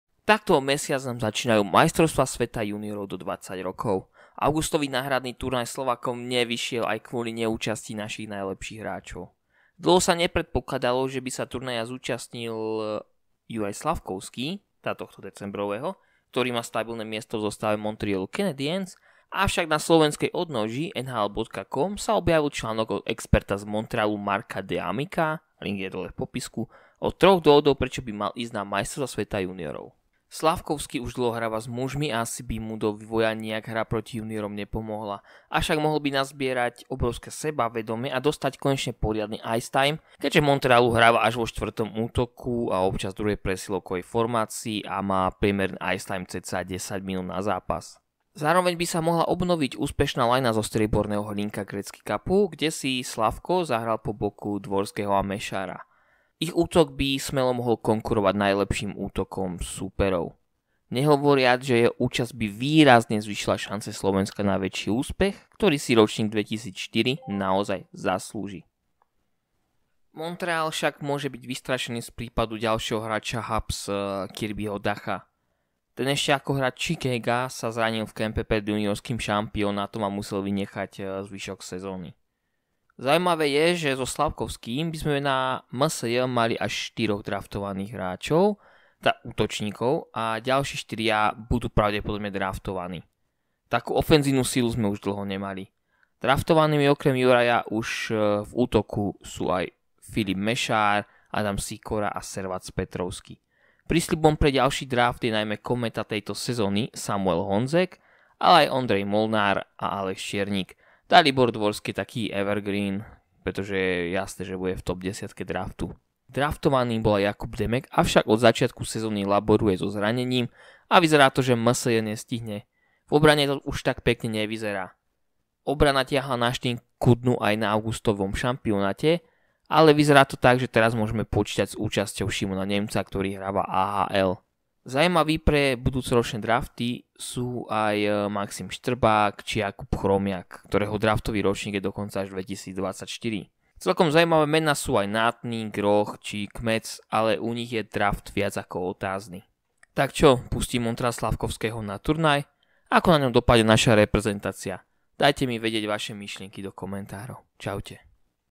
Pôjde Slafkovský na Juniorský šampionát?
Pôjde draftová jednotka Juraj Slafkovský na MSJ 2023?